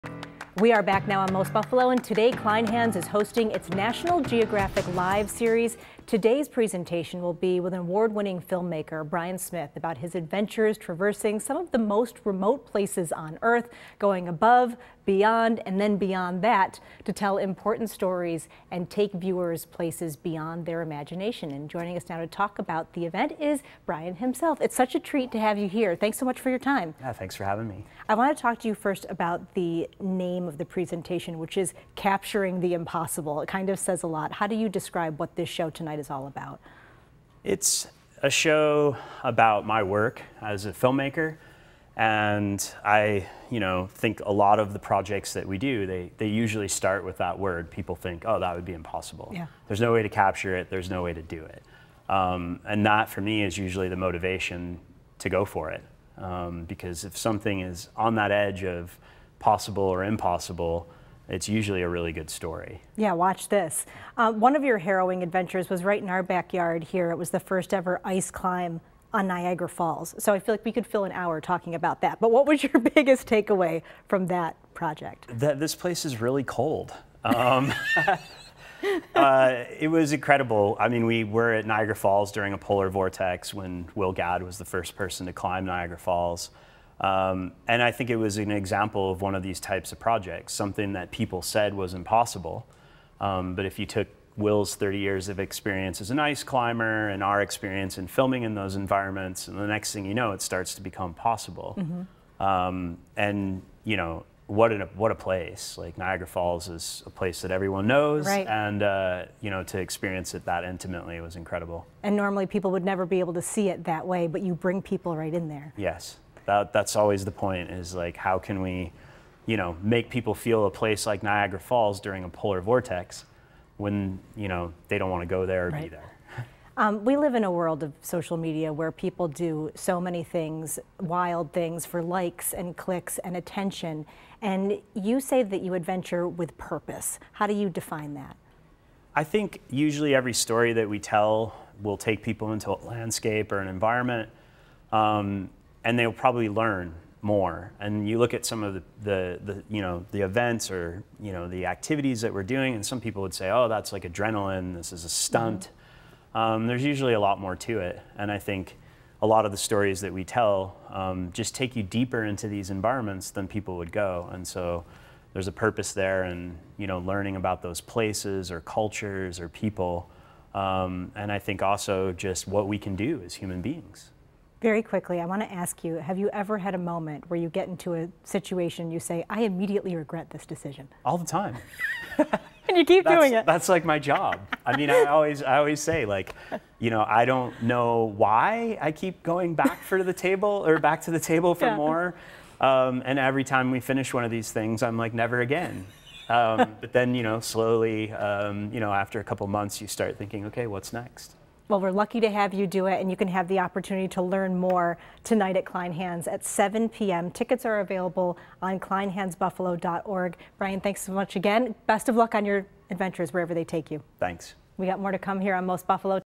National Geographic's Bryan Smith visits Most Buffalo
National Geographic live series at Kleinhans